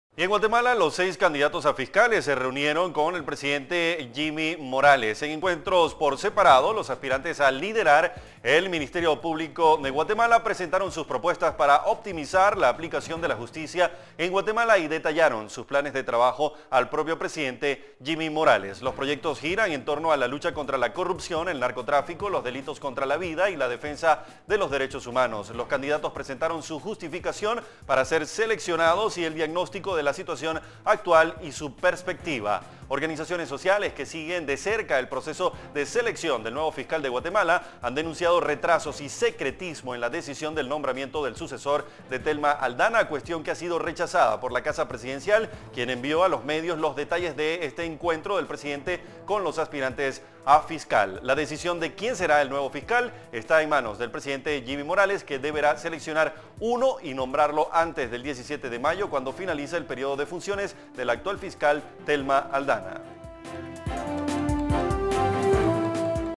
Presidente de Guatemala deberá designar a nuevo fiscal general antes del 17 de mayo
En Guatemala los seis candidatos a fiscales se reunieron con el presidente Jimmy Morales.

En encuetros por separado los aspirantes a liderar el Ministerio Público de Guatemala, presentaron sus propuestas para optimizar la aplicación de la justicia en Guatemala y detallaron sus planes de trabajo al propio presidente Jimmy Morales.

Los proyectos giran en torno a la lucha contra la corrupción, el narcotráfico , los delitos contra la vida y la defensa de los derechos humanos, los candidatos presentaron su justificación para ser seleccionados y el diagnóstico de la situación actual y perspectiva.

Organizaciones sociales que siguen de cerca el proseco de selección del nuevo fiscal en Guatemala, han denunciado retrasos y secretismo en la decisión  del nombramiento del sucesor de Thelma Aldana, cuestión que ha sido rechazada por la casa presidencial quien envió a los medios los detalles del encuentro del presidente con los aspirantes a fiscal.

La decisión de quién será el nuevo fiscal está en manos del presidente Jimmy Morales que deberá seleccionar uno y mombrarlo antes del 17 de mayo, cuando finaliza el periodo de funciones de la actual fiscal Thelma Aldana.

Redacción / CB24